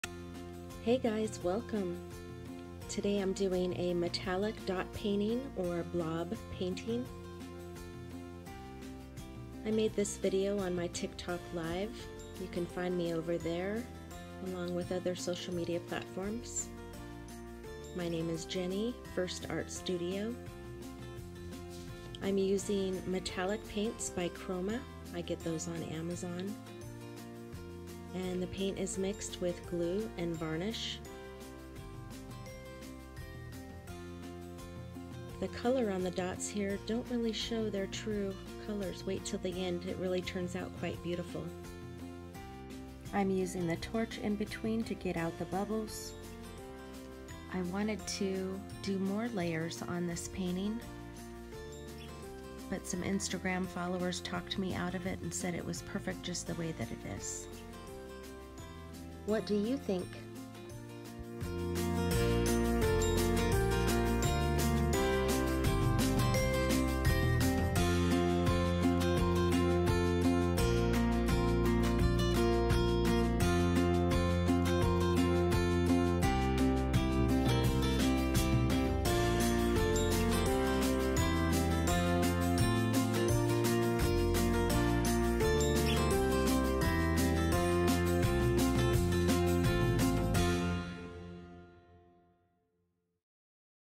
Metallic Dot Blob Painting on Wood Canvas
This is a metallic dot blob painting I did on my TikTok live. I used chroma metallic paints that I got on Amazon in every color they offer; three golds, copper, silver and bronze. This is a heavier paint than say Decoart metallics and dry nicely. I mix the paint with glue and varnish. I use the torch in between to get out the bubbles. I prepped the canvas with gesso, then painted it black and white. The dots when wet are a different color than when they are dried. The end result is beautiful.